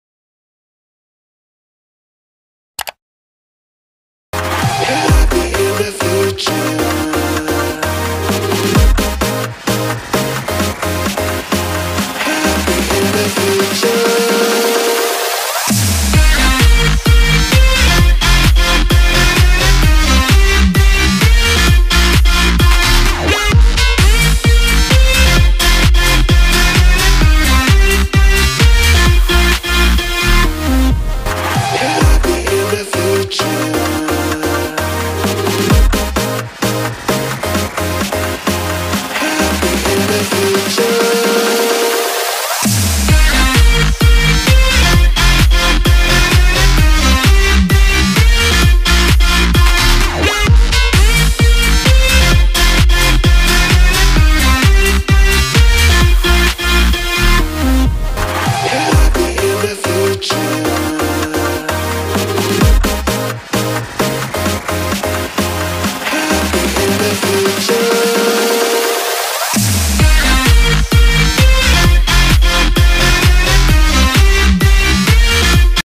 Rumah kami tidak besar tapi bahagia bersama#cat
kucing-kucing Yang diadopsi hidup bahagia bersama